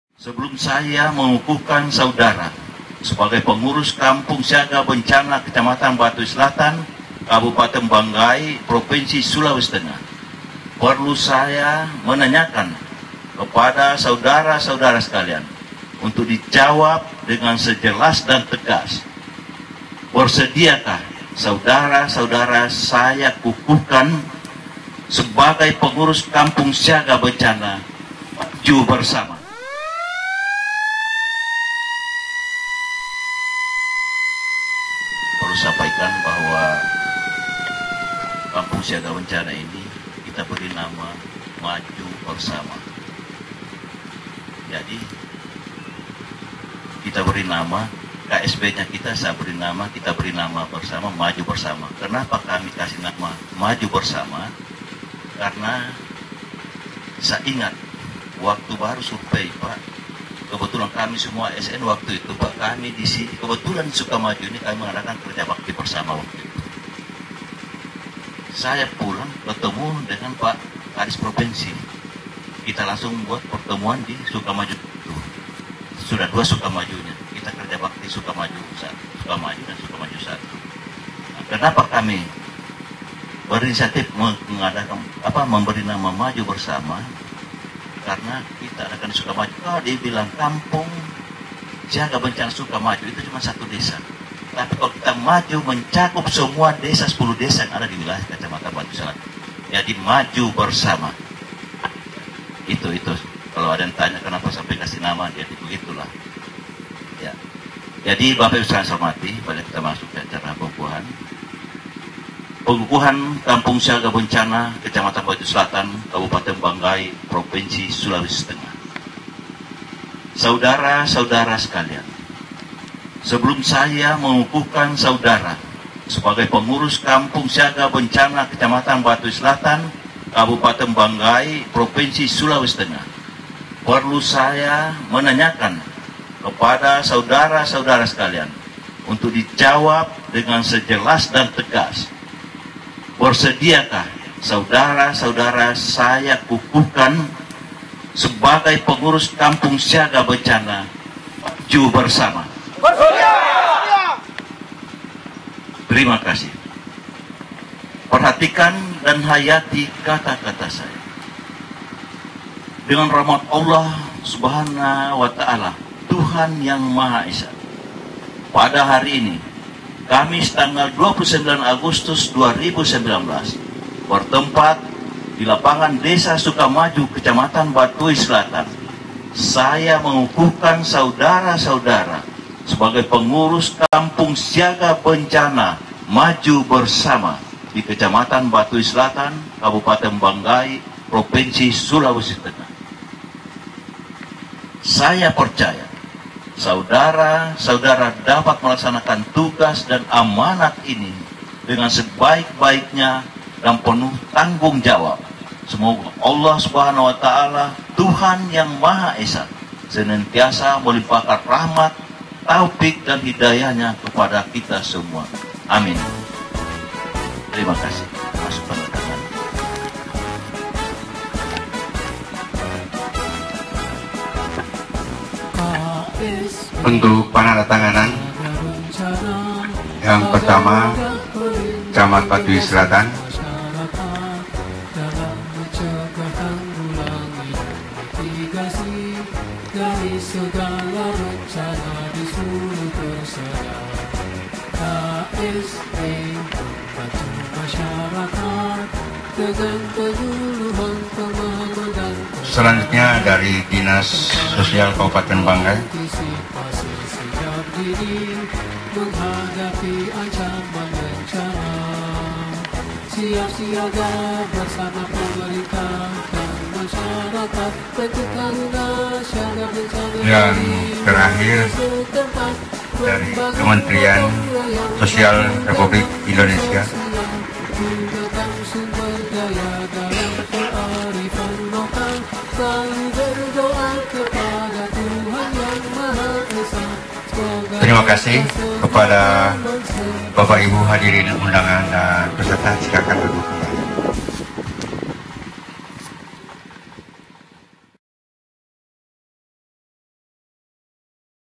PENGUKUHAN KSB MAJU BERSAMA KEC. BATUI SELATAN KAB. BANGGAI SULAWESI TENGAH [HD]
Pengukuhan Kampung Siaga Bencana (KSB) Kecamatan Batui Selatan Kabupaten Banggai Sulawesi Tengah dilaksanakan tanggal 29 Agustus 2019 di Desa Sukamaju.